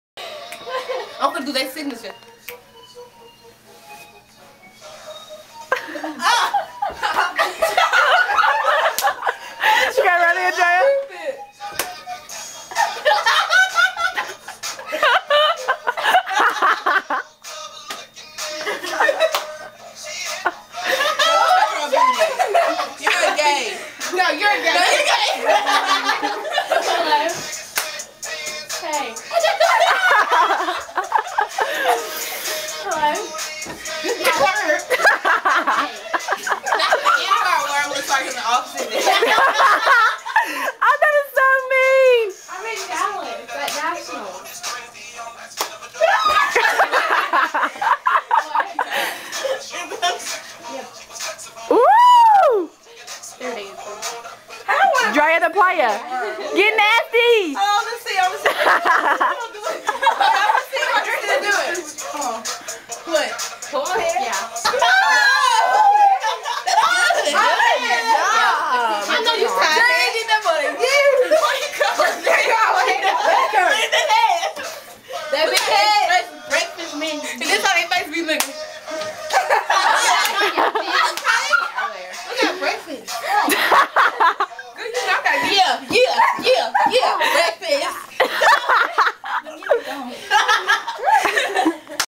ADTS Nationals-HOTEL TIME
we were so bored [waiting on ms high kick competition] so we decided to jam out in the hotel room. sooo FUNNY!
paige is on the left and im [haley] on the right!...